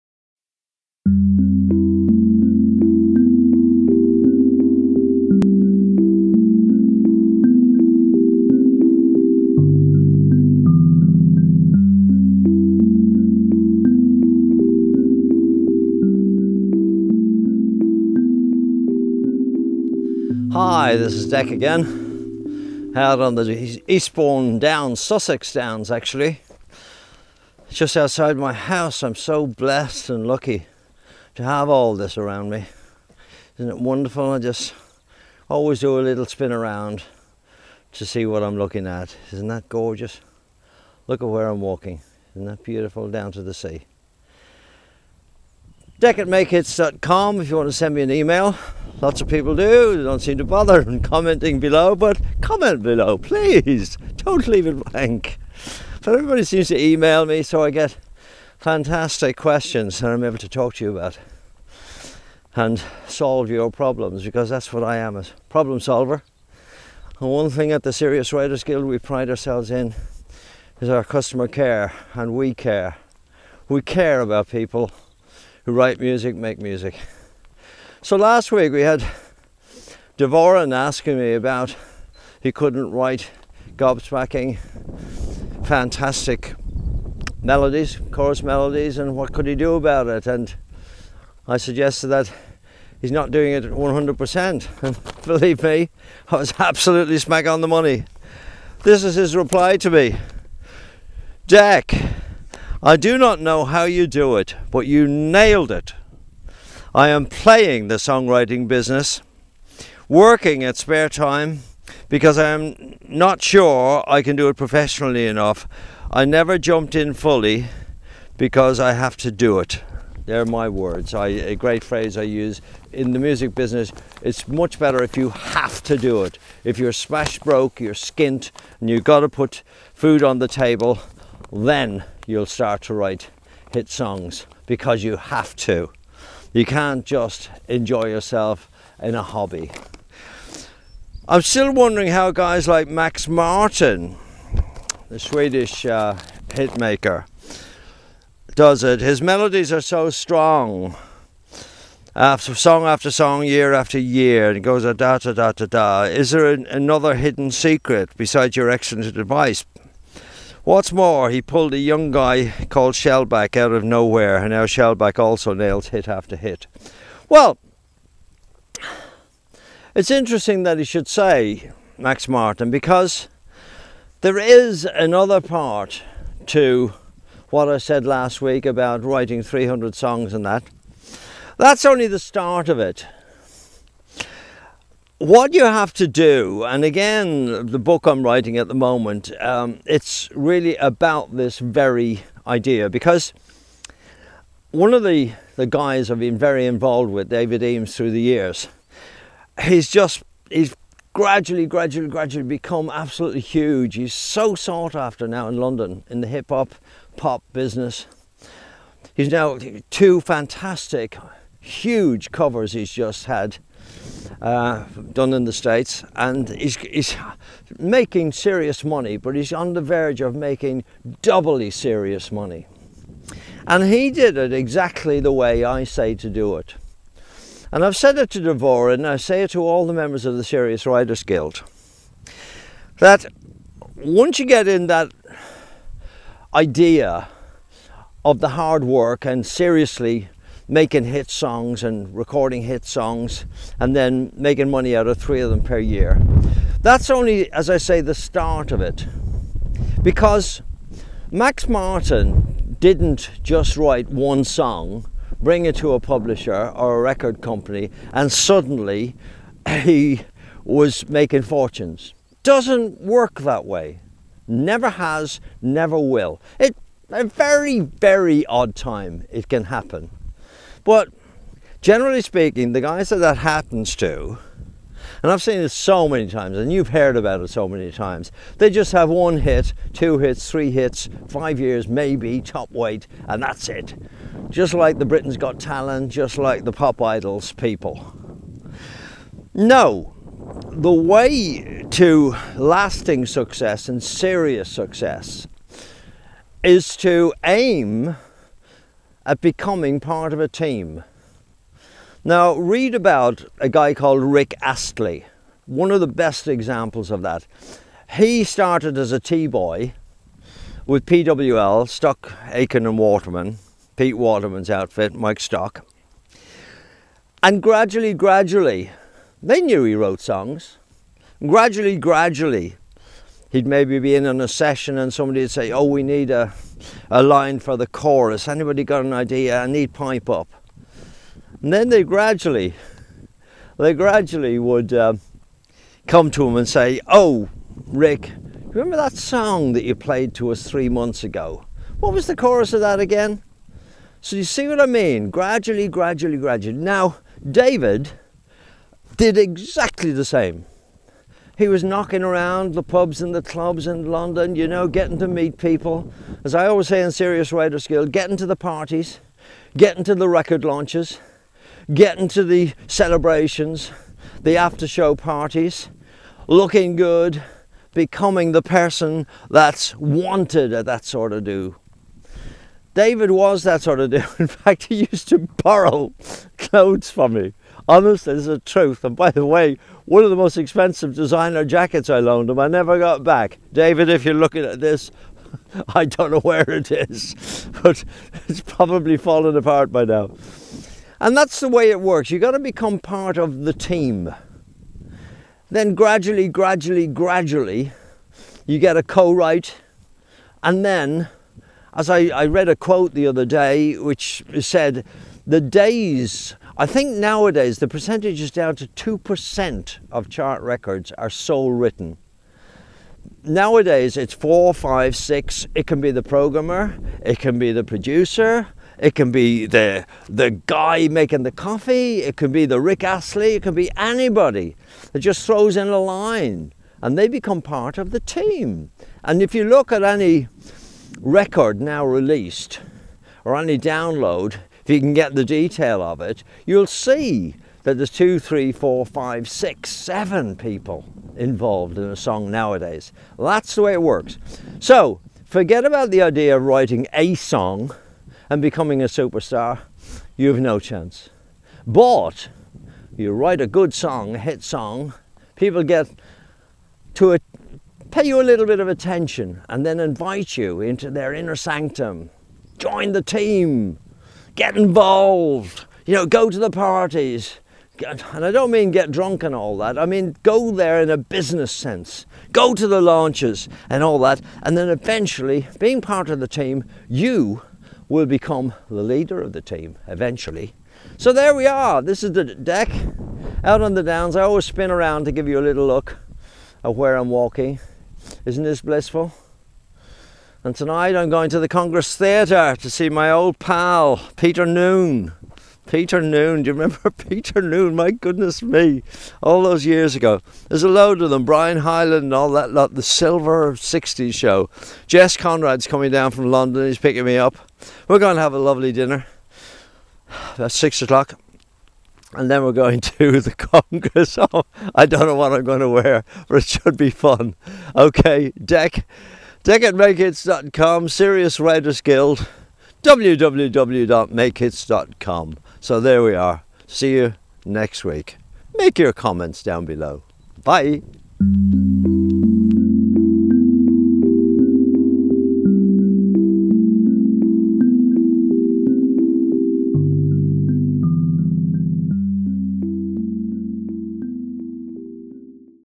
4 minutes 2016 season No 22 ‘I Cannot Compose Quality Chorus Melodies - PART TWO!’
It's Sunday ... it's 7pm ... it's '4 Minutes on the Net with Dec' ... 2016 SEASON NO. 22 ...  ‘I Cannot Compose Quality Chorus Melodies - PART TWO!’  … Dec explains the way it’s done …. you just join the team…   get noticed   … a team can be up to eight writers/producers/engineers ….

to see ‘How To Make a $Million From Your Music’